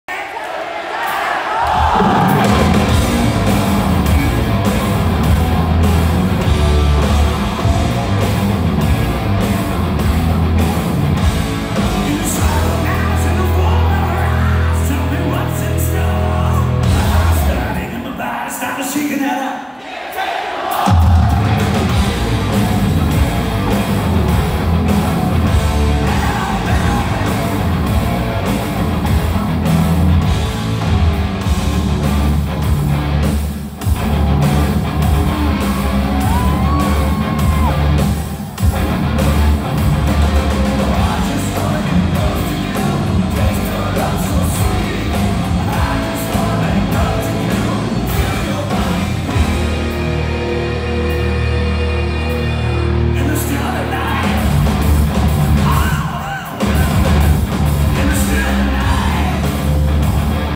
Decrepit rocker April 25, 2019 Hollywood, Florida
Decrepit rocker at the Seminole Hard Rock Hotel and Casino